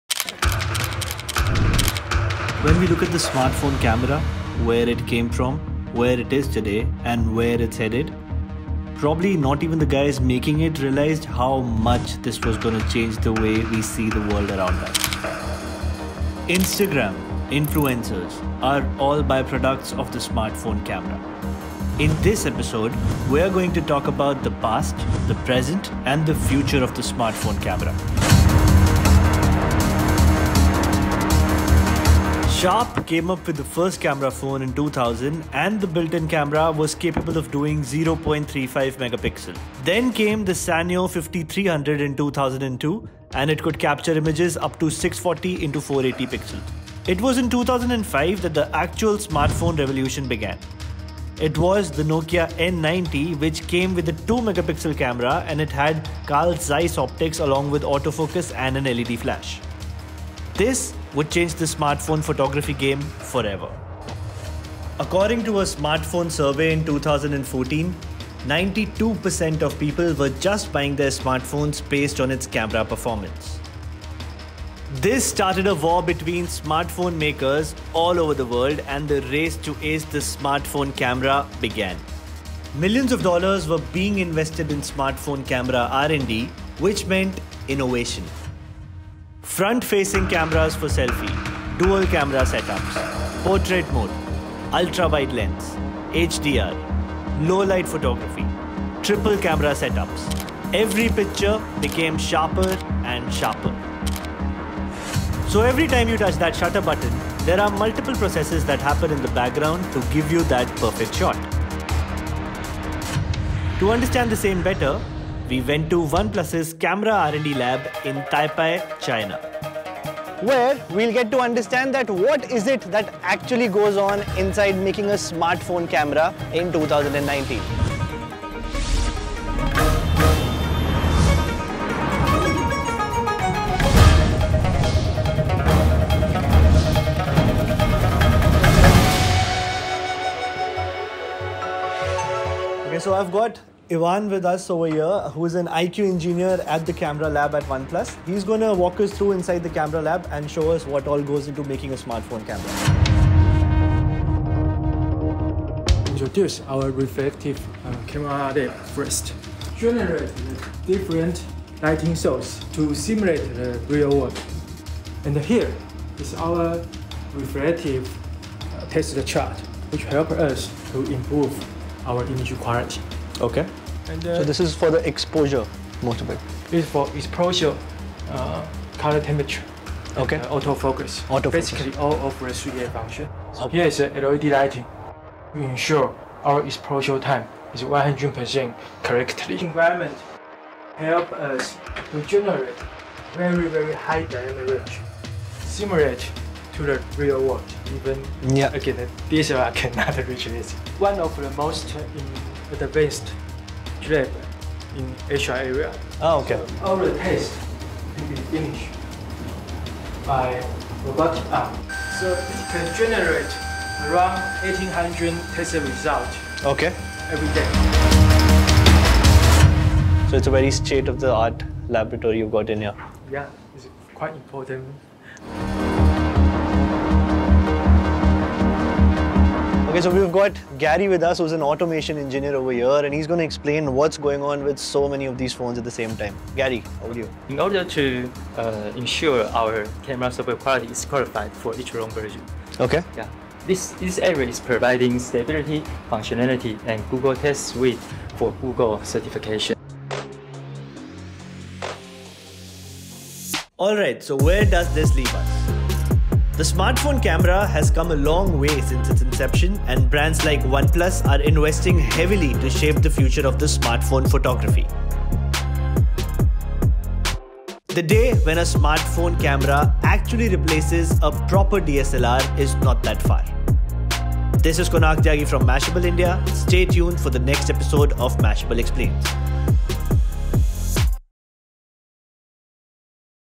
Evolution of the Smartphone Camera
Smartphone cameras have come a long way since the last two decades. In this episode of Mashable Explains, we try to break down the past, the present and the future of Smartphone photography.